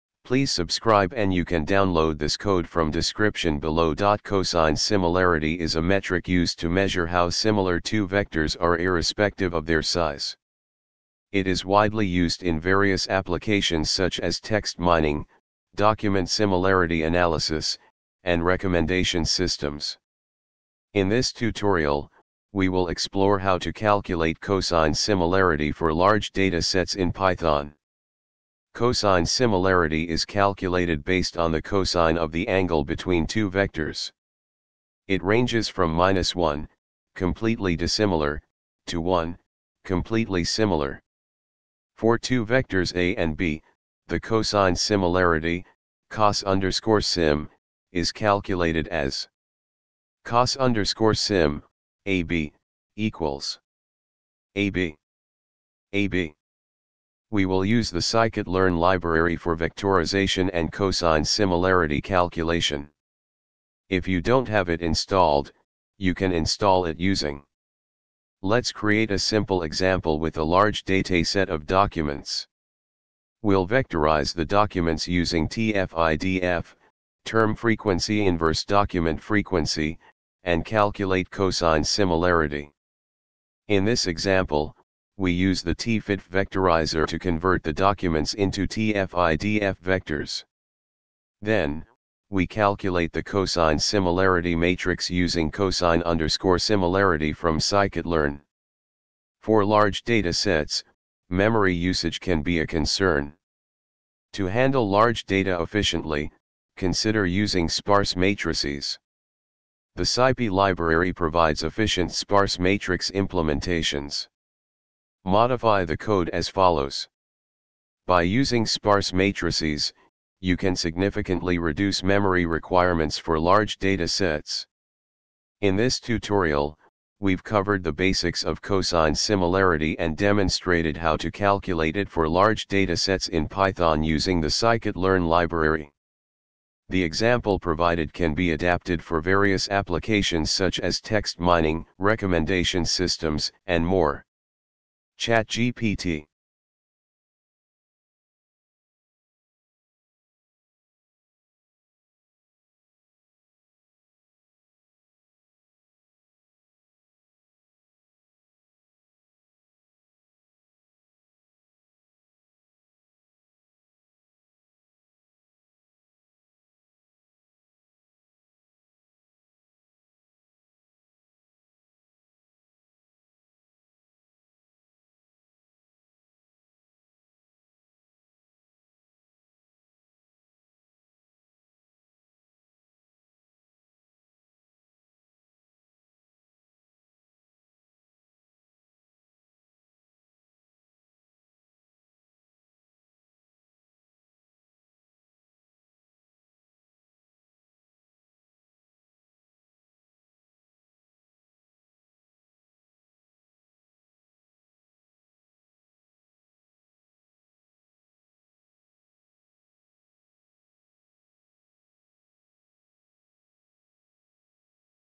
cosine similarity of large data in python
Cosine similarity is a metric used to measure how similar two vectors are irrespective of their size. It is widely used in various applications such as text mining, document similarity analysis, and recommendation systems. In this tutorial, we will explore how to calculate cosine similarity for large data sets in Python.
Cosine similarity is calculated based on the cosine of the angle between two vectors. It ranges from -1 (completely dissimilar) to 1 (completely similar). For two vectors A and B, the cosine similarity (cos_sim) is calculated as:
cos_sim(A,B)=
∥A∥⋅∥B∥
A⋅B
We will use the scikit-learn library for vectorization and cosine similarity calculation. If you don't have it installed, you can install it using:
Let's create a simple example with a large dataset of documents. We'll vectorize the documents using TF-IDF (Term Frequency-Inverse Document Frequency) and calculate cosine similarity.
In this example, we use the TfidfVectorizer to convert the documents into TF-IDF vectors. Then, we calculate the cosine similarity matrix using cosine_similarity from scikit-learn.
For large datasets, memory usage can be a concern. To handle large data efficiently, consider using sparse matrices. The scipy library provides efficient sparse matrix implementations. Modify the code as follows:
By using sparse matrices, you can significantly reduce memory requirements for large datasets.
In this tutorial, we've covered the basics of cosine similarity and demonstrated how to calculate it for large datasets in Python using the scikit-learn library. The example provided can be adapted for various applications such as text mining, recommendation systems, and more.
ChatGPT